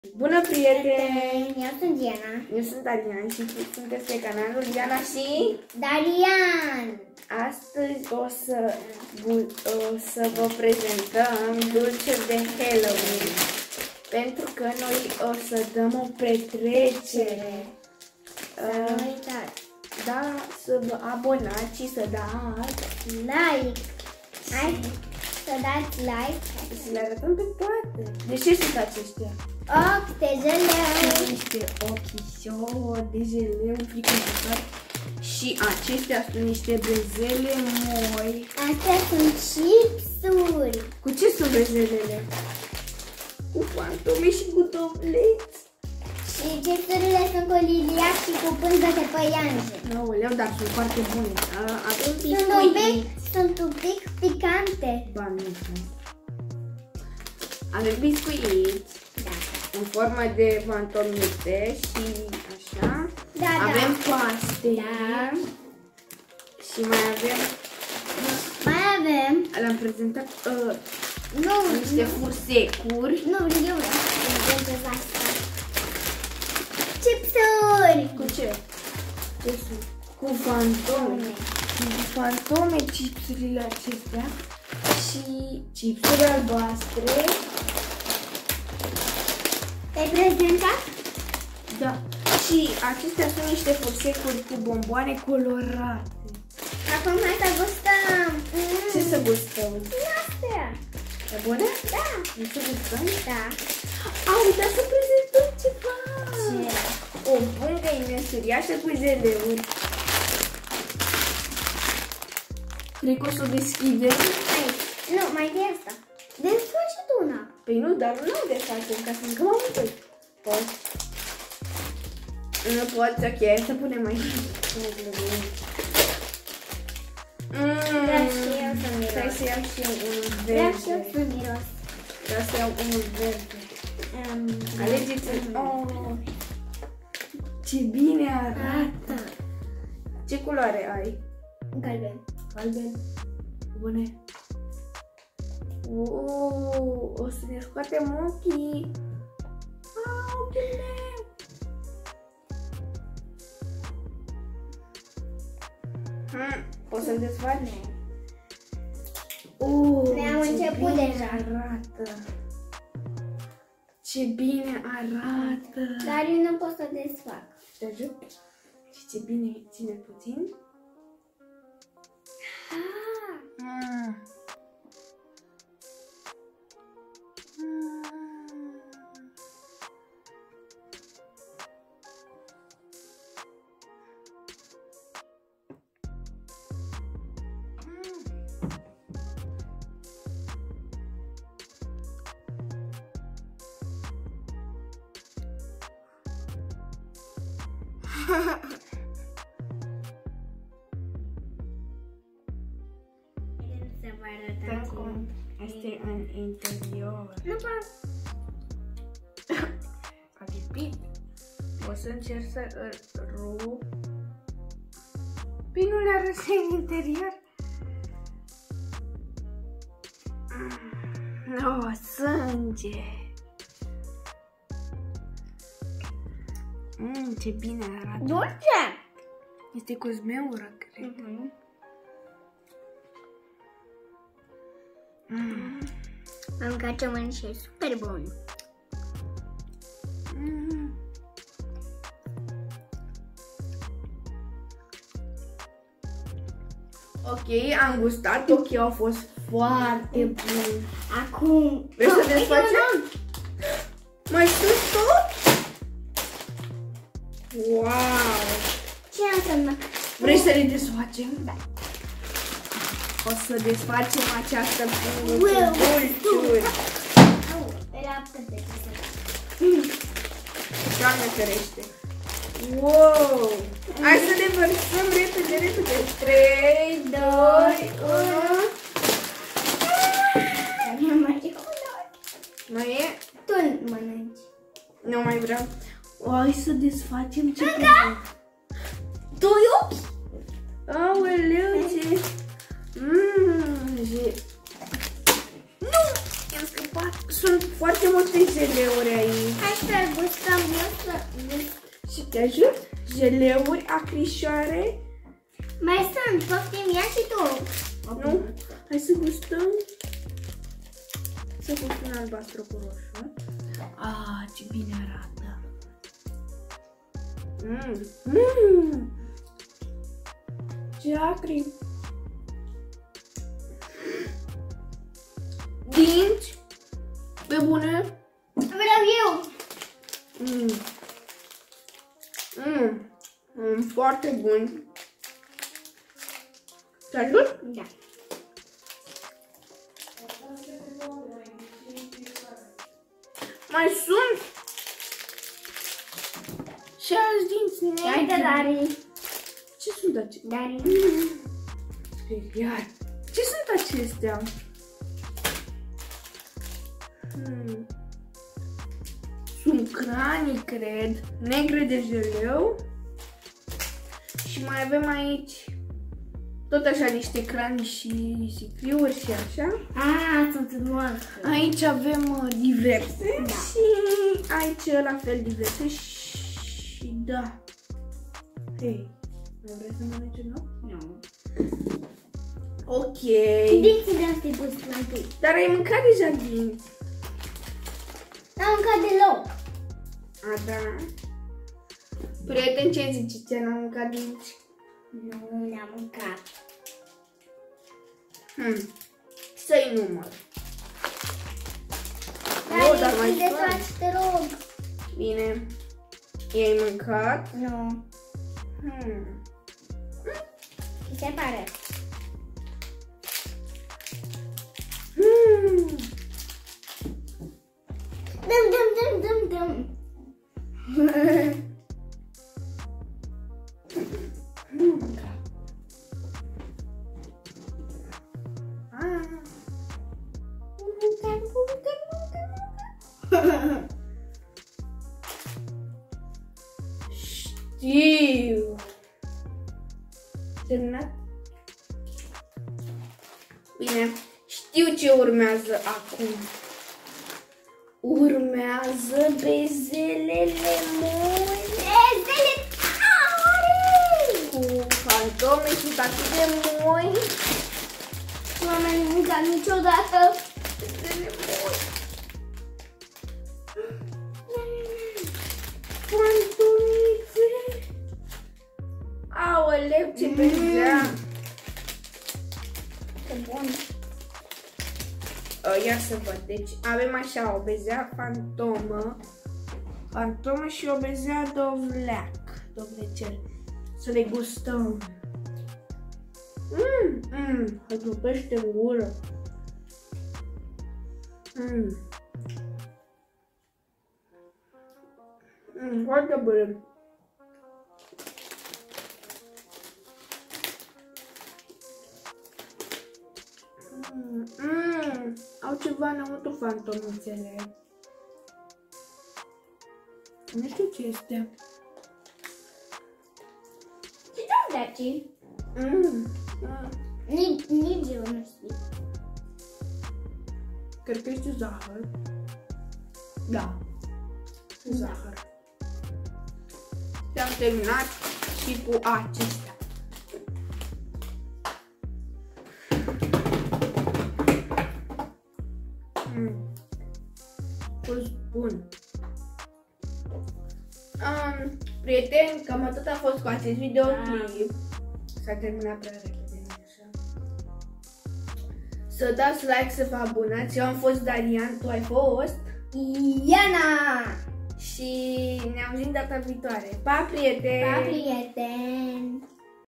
HALLOWEEN - Taste Test Dulciuri Party 👻🎃🎊🍬🍭halloween
Continuam pregătirile pentru Halloween Party și am. fost la Lidl să cumpărăm dulciuri specifice acestei sărbători 🎃👻